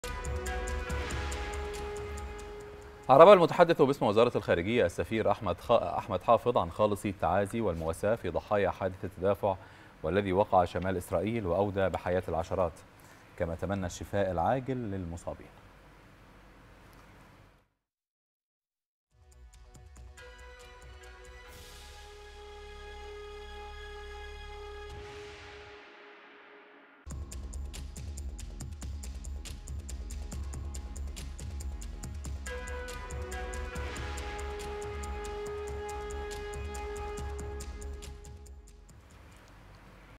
المتحدث باسم الخارجية يُعرِب عن خالص التعازي في ضحايا حادث التدافع بدولة الاحتلال الإسرائيلي
المتحدث باسم الخارجية: خالص التعازي في ضحايا  حادث التدافع بشمال إسرائيل والشفاء للمصابين